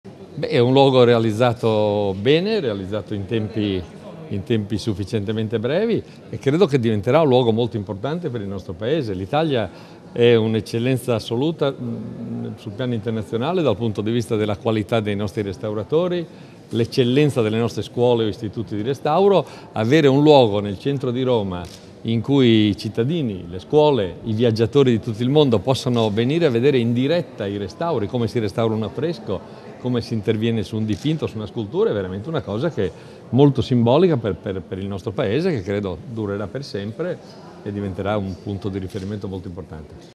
Apre Restauro Aperto, Franceschini: "Un luogo simbolo"
(Agenzia Vista) - Roma, 24 Ottobre 2017 - Il ministro delle Attività Culturali e del Turismo, Dario Franceschini, ha inaugurato il laboratorio di "Restauro aperto" dell’Istituto superiore per la conservazione e il restauro, presso la chiesa di Santa Marta al Collegio Romano a Roma. Gli spazi della chiesa sono stati allestiti come laboratorio di restauro e aperti alla libera visione del pubblico.